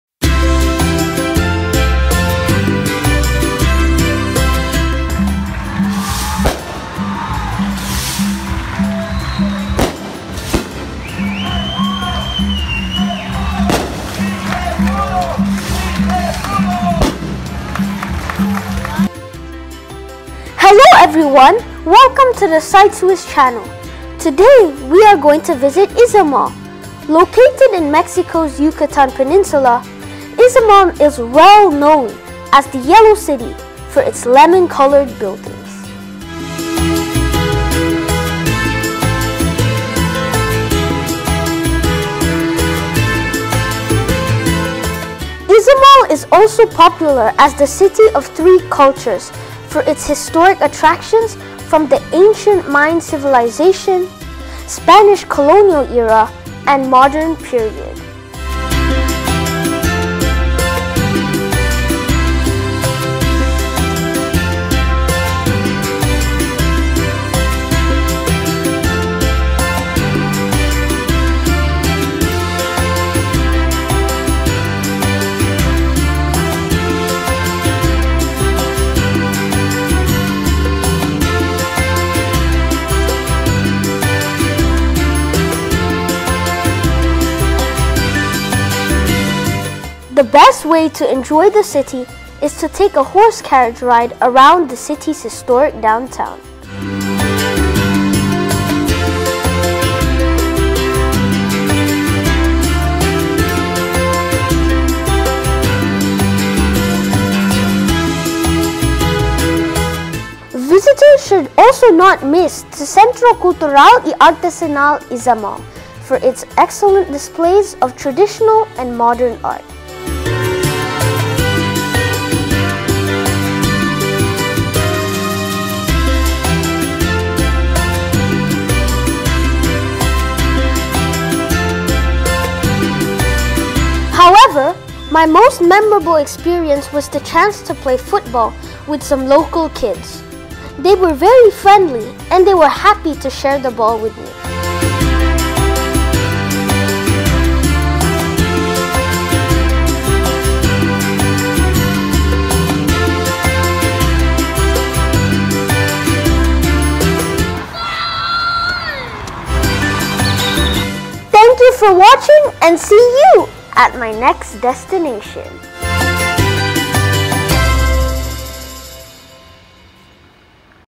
IZAMAL: Mexico's Yellow City
Located in the Yucatan Peninsula, Izamal well-known for its yellow-painted buildings and historic attractions from the Mayan civilization and Spanish colonial period.